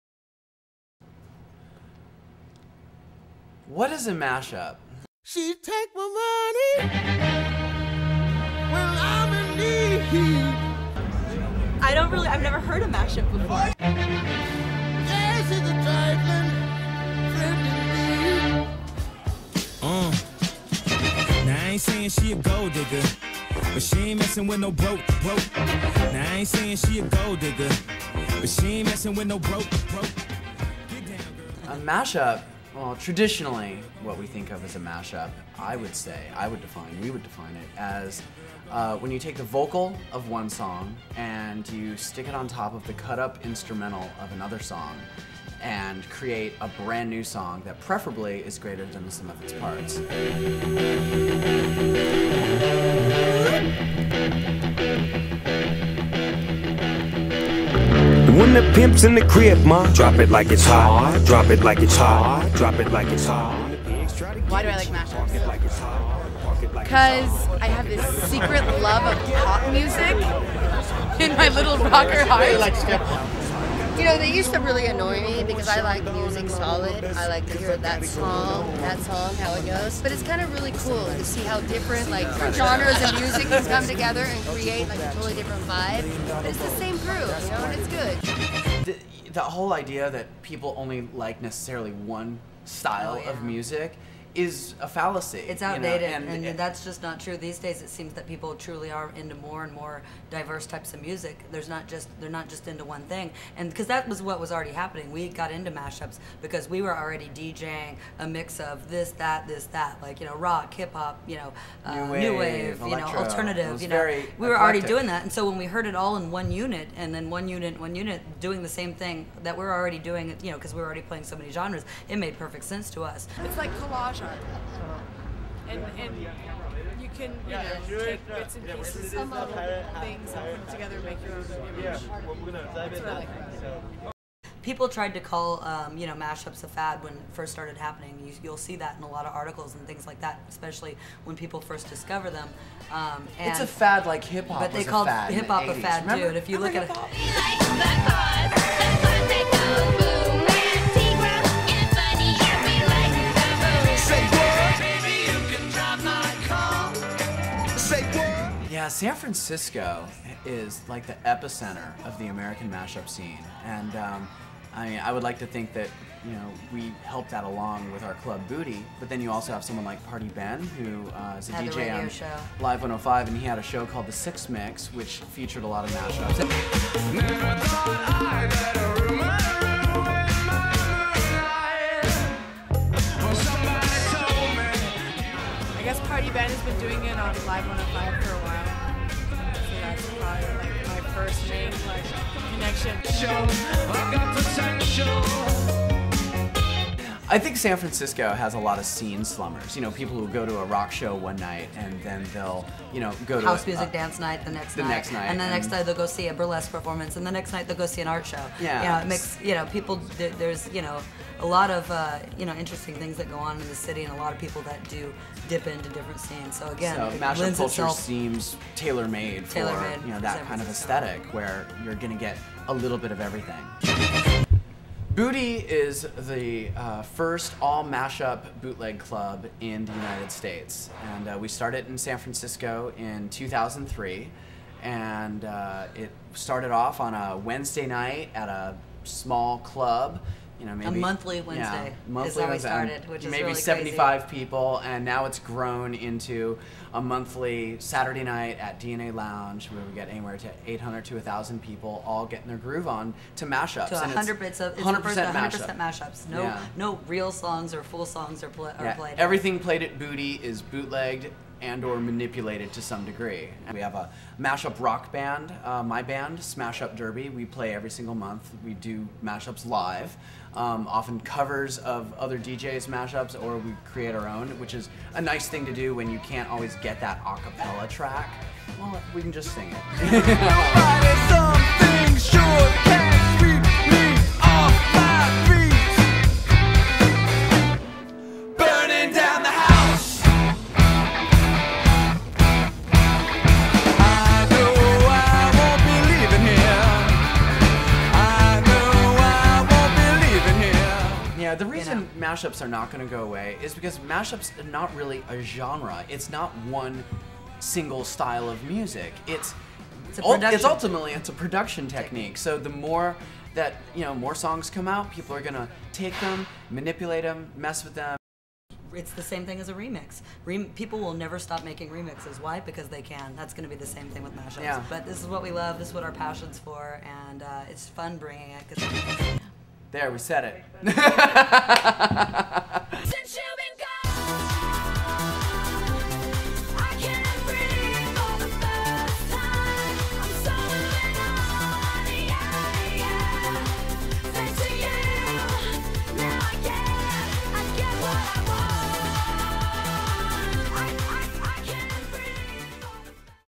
Digital Bootleggers
The beginnings of a documentary about the mash-up scene in San Francisco